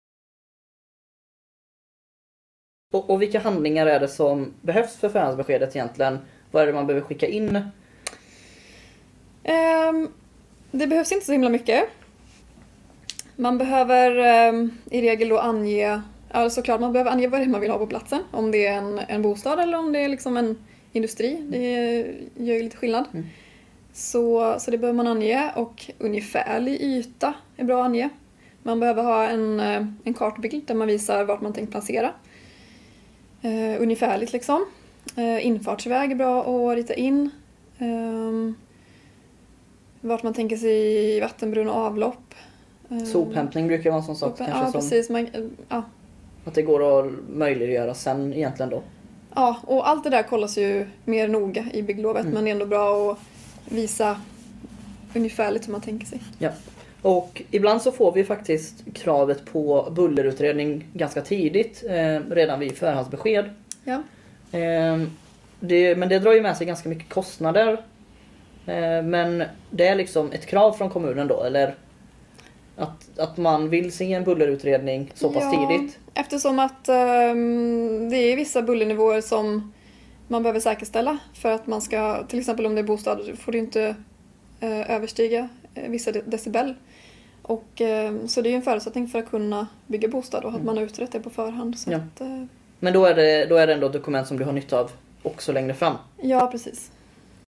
Detta Krävs Inför Ditt Förhandsbesked
7 Misstag Att Undvika När Du Söker Bygglov

LADDA NER GRATIS PDF

I dagens video berättar @LT Ingenjörsbyrå om vilka handlingar som krävs för ett förhandsbesked och vad som är bra att tänka på.

00:00 Vilka handlingar krävs för att ansöka om Förhandsbesked?

🍿 Besök vår kanal och se alla våra filmer!